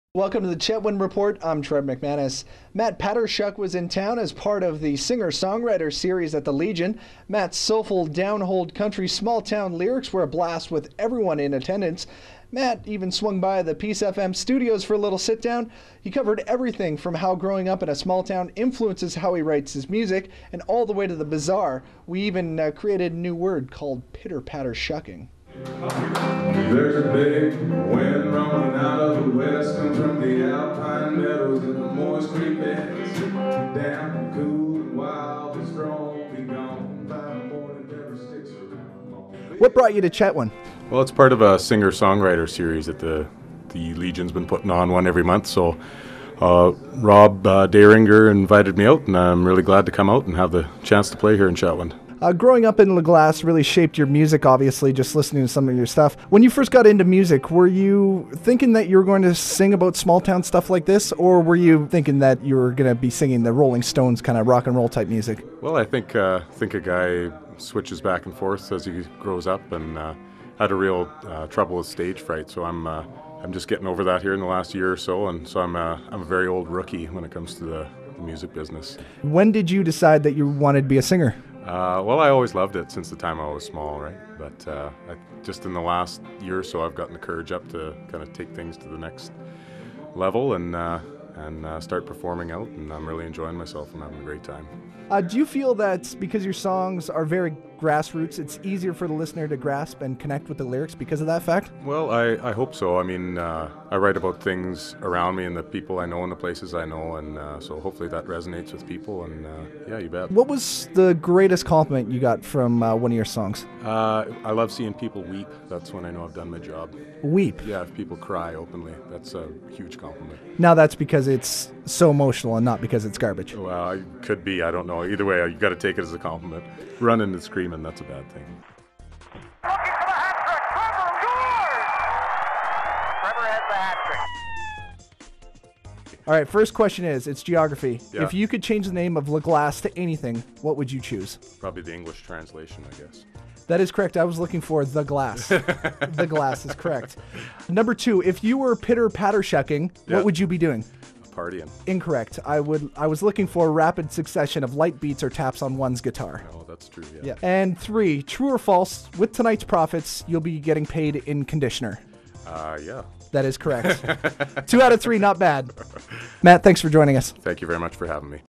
Matt Patershuk in Chetwynd
Singer Songwriter Matt Patershuk entertained the people of Chetwynd at the Legions Singer Songwriter Night.